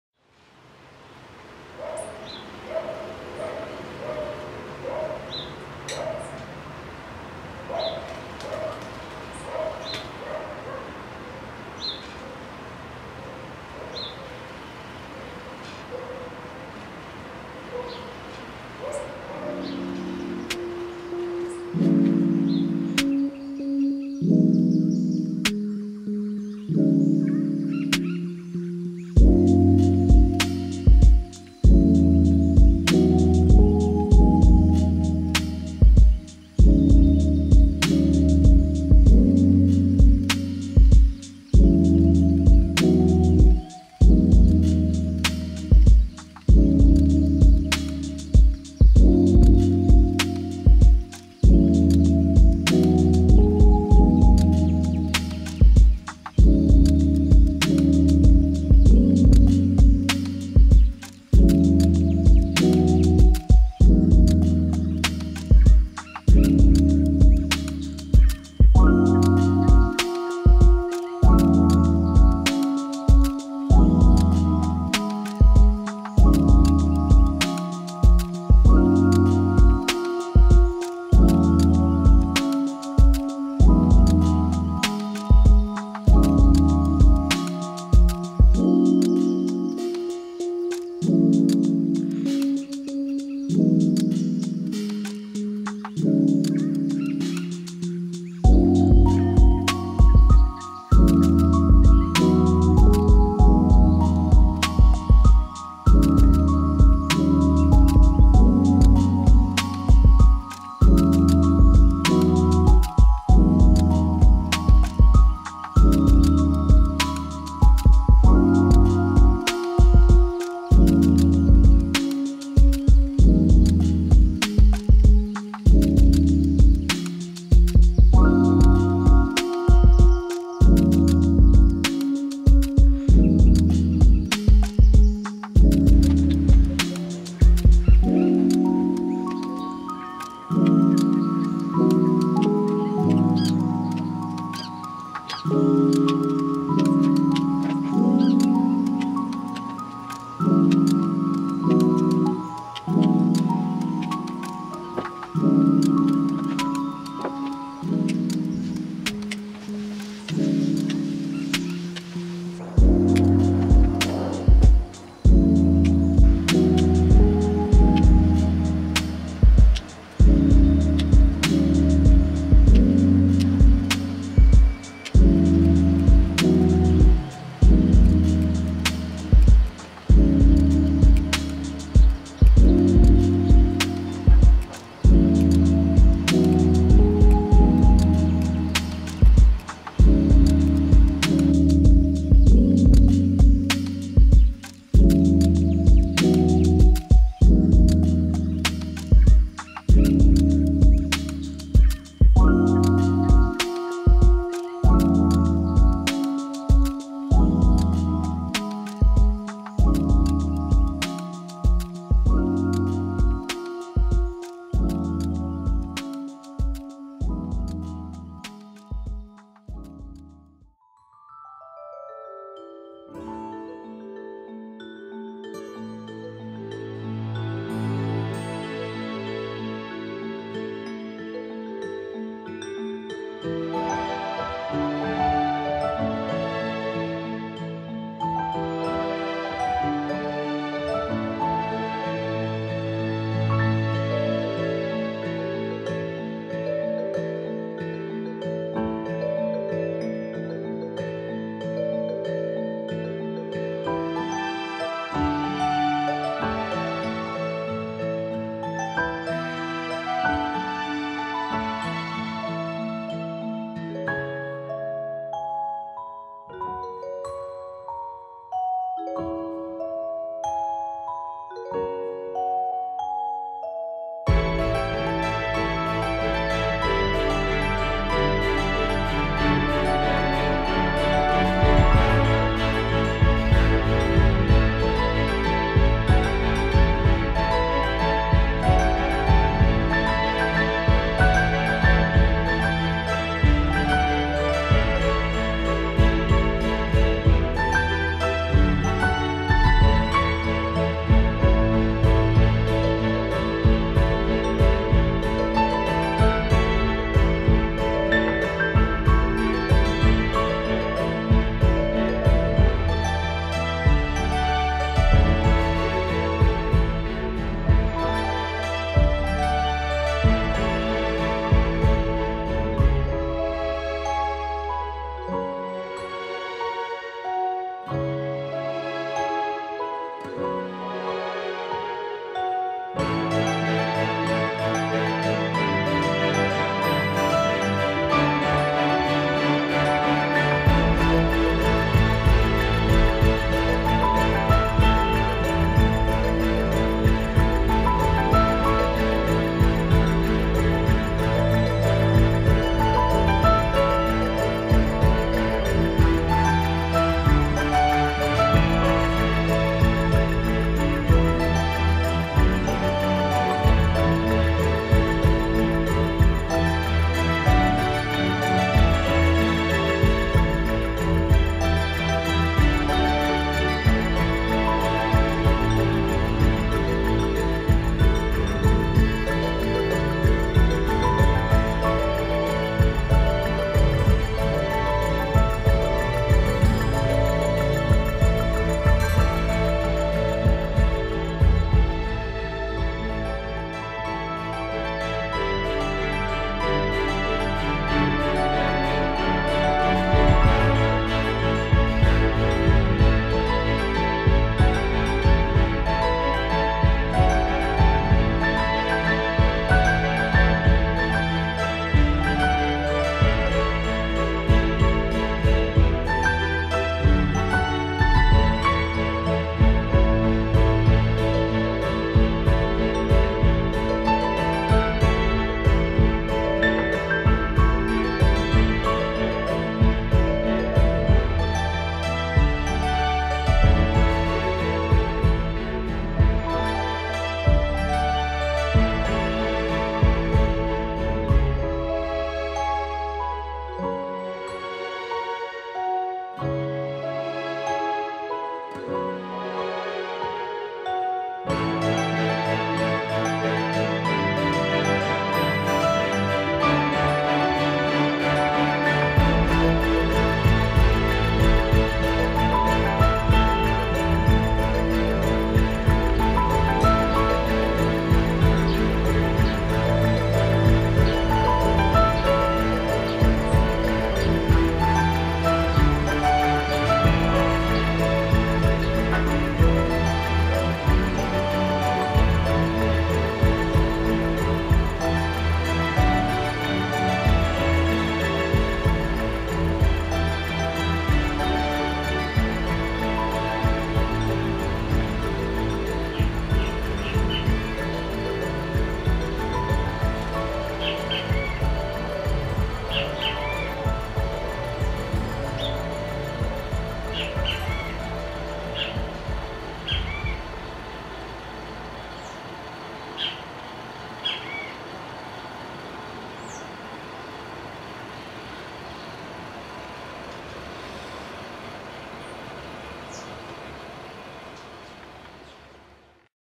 Vale das Azaleias - penjing (vídeo 3) atualização
este é o terceiro vídeo do Vale das Azaleias, deixamos a estética para as próximas intervenções para desfrutar das belas flores que vieram em vastas brotações...